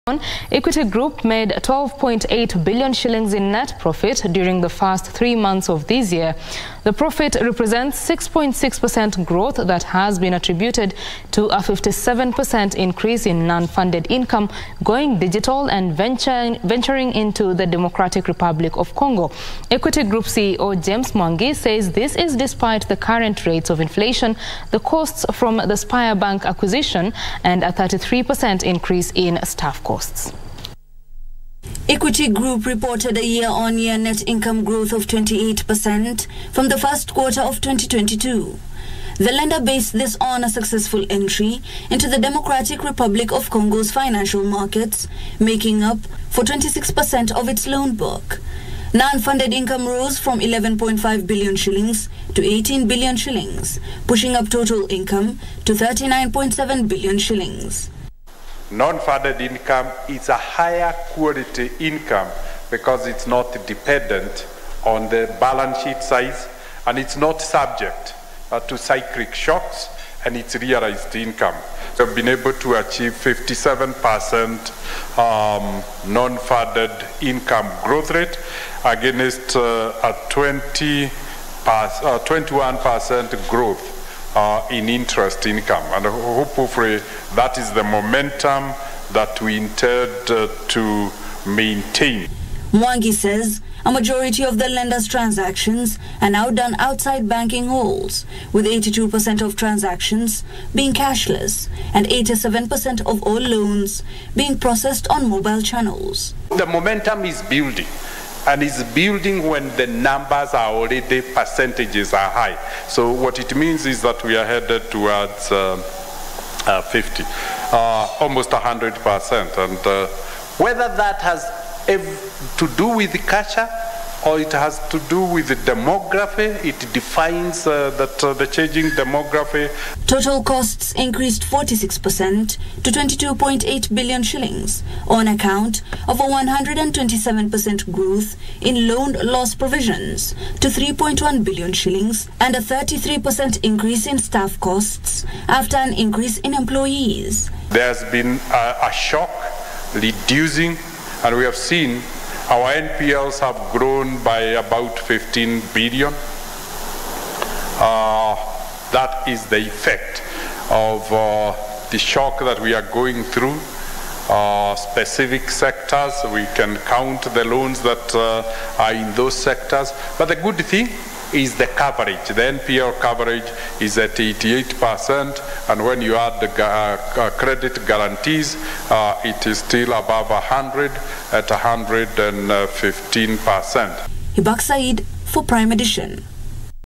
Equity first quarter wins a 28% income growth
Equity Group made 12.8 billion shillings in net profit during the first three months of this year. The profit represents 6.6% growth that has been attributed to a 57% increase in non-funded income, going digital and venture into the Democratic Republic of Congo. Equity Group CEO James Mwangi says this is despite the current rates of inflation, the costs from the spire bank acquisition and a 33% increase in staff costs.